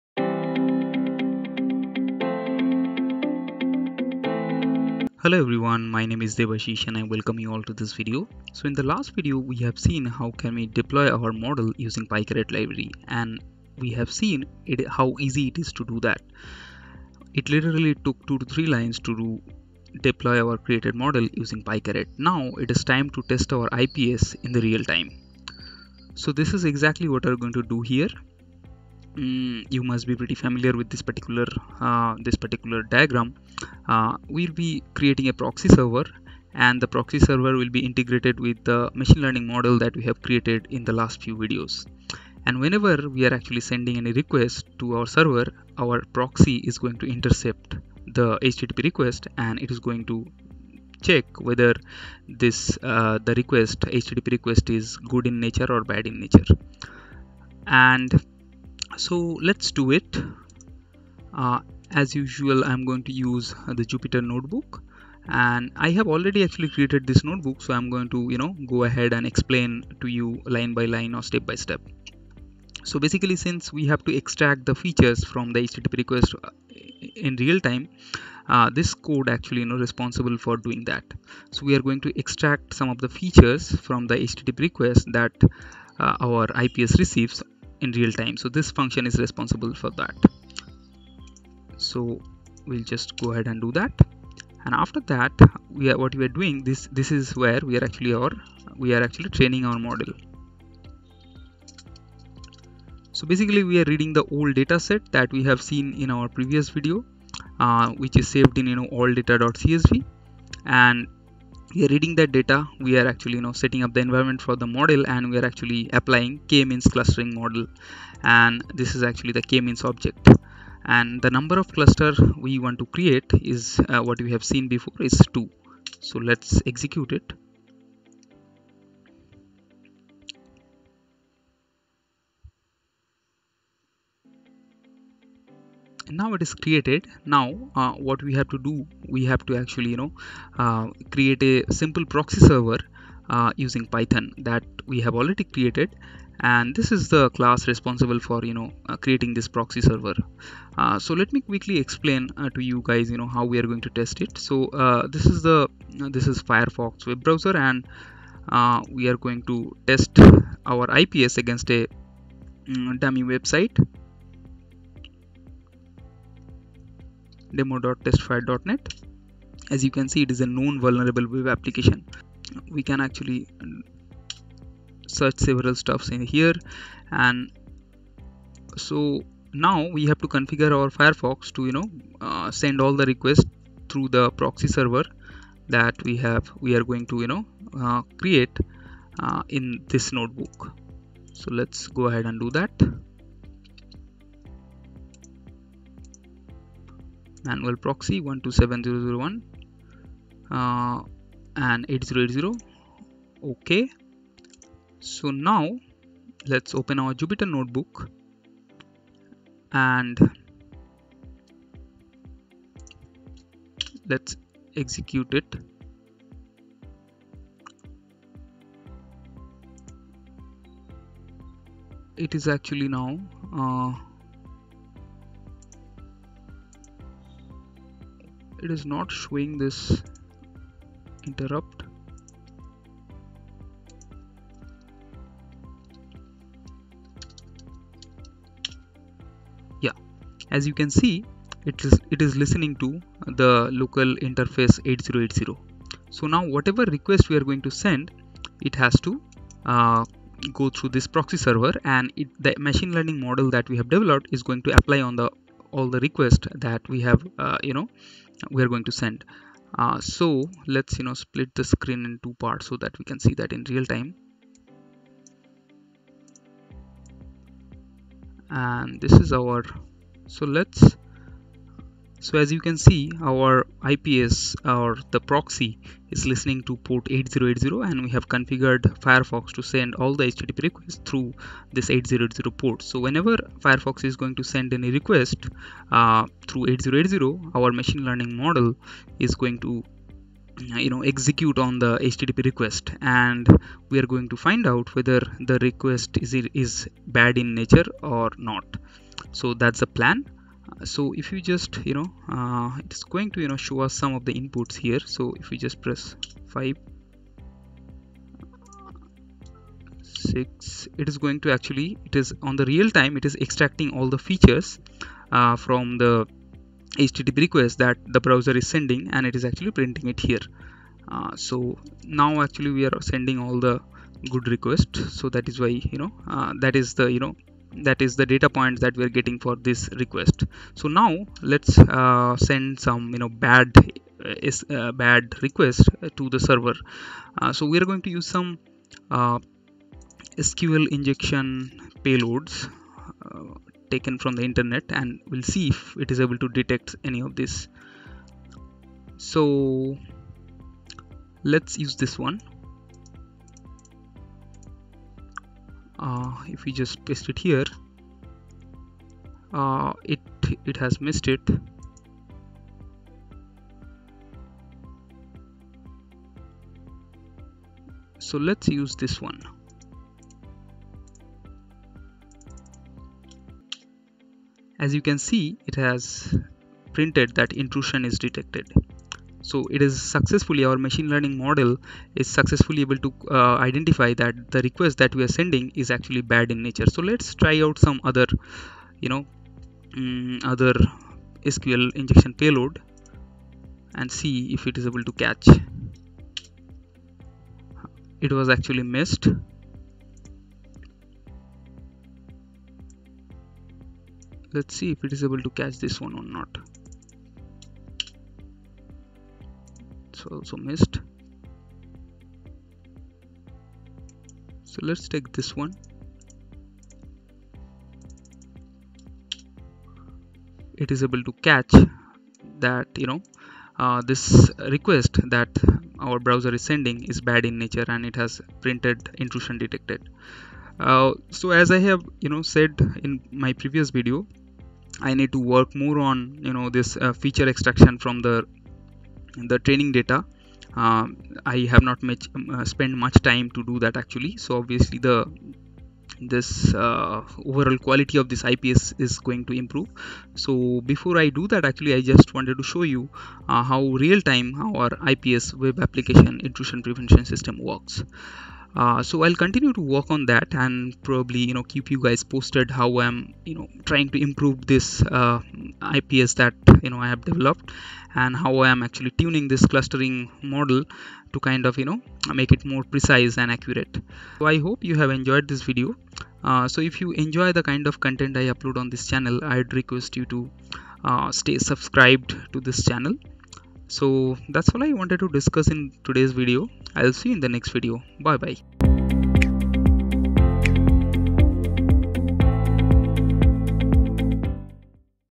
Part 8/8: ML Based Web App Firewall : Testing the IPS in Real Time
Recently I started to play around with some machine learning stuffs, and I decided to build a small project related to cyber security. So, I decided to build a Machine Learning based Web Application firewall.